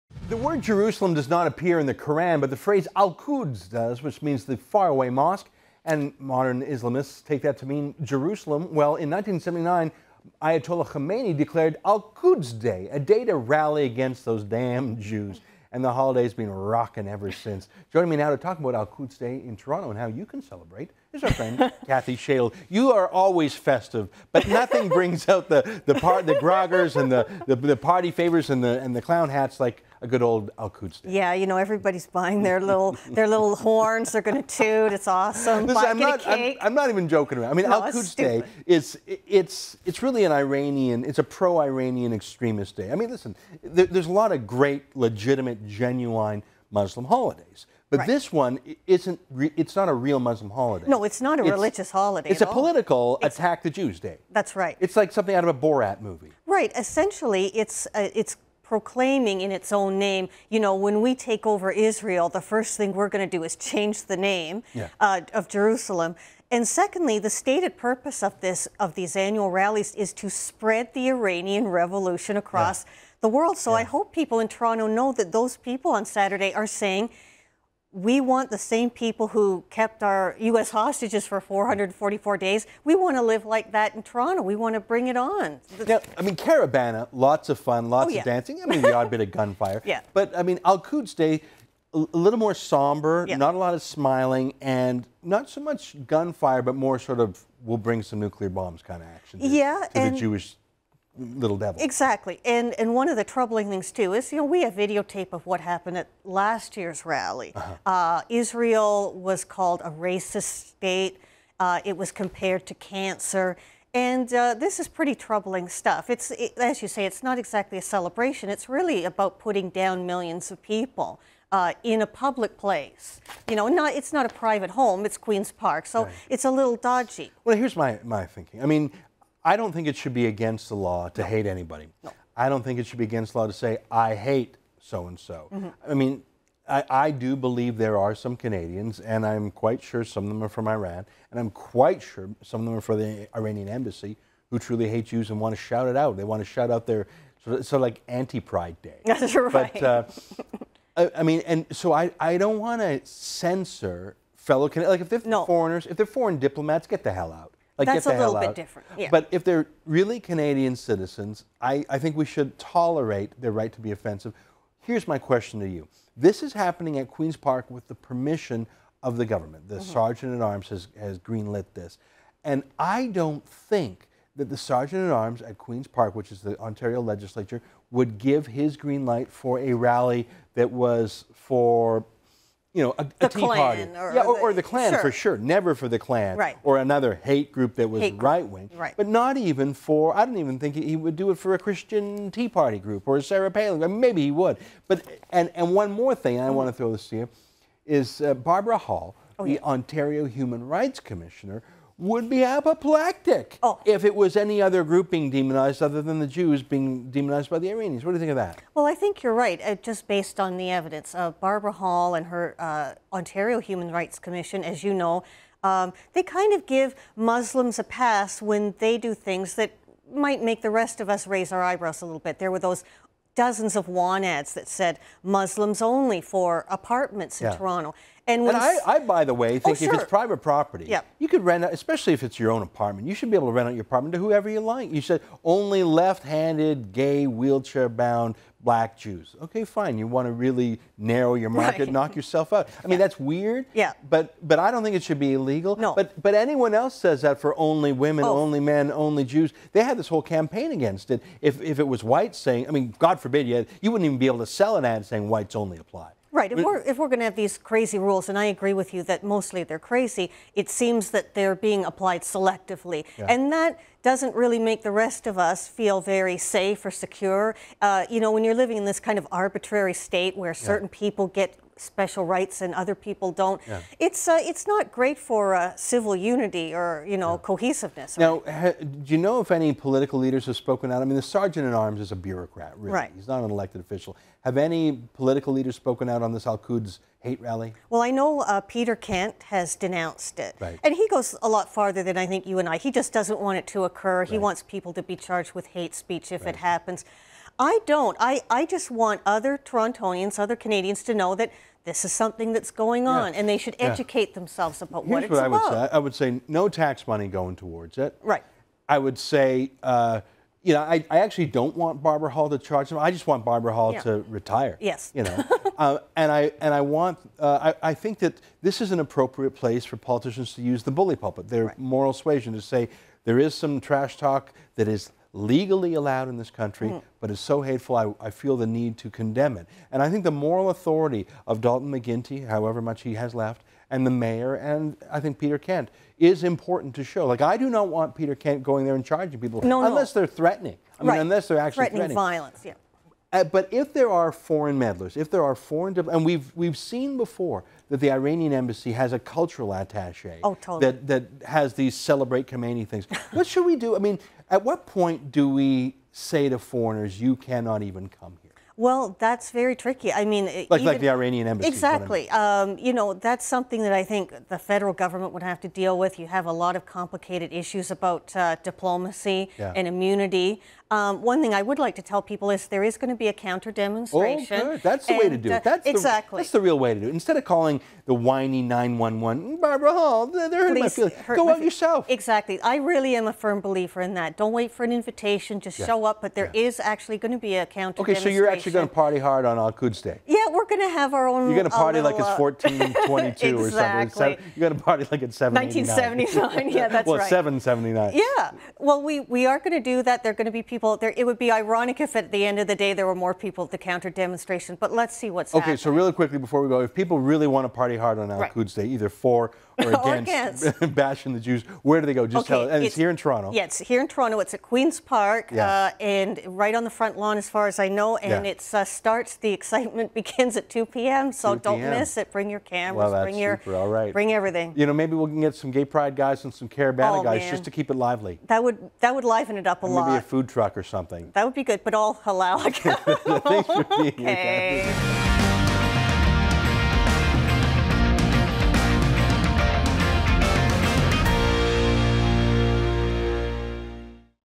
Opposing Al-Quds
Kathy Shaidle from Five Feet of Fury joins Ezra Levant to talk about how she and some like minded people plan to protest the Al-Quds Day rally.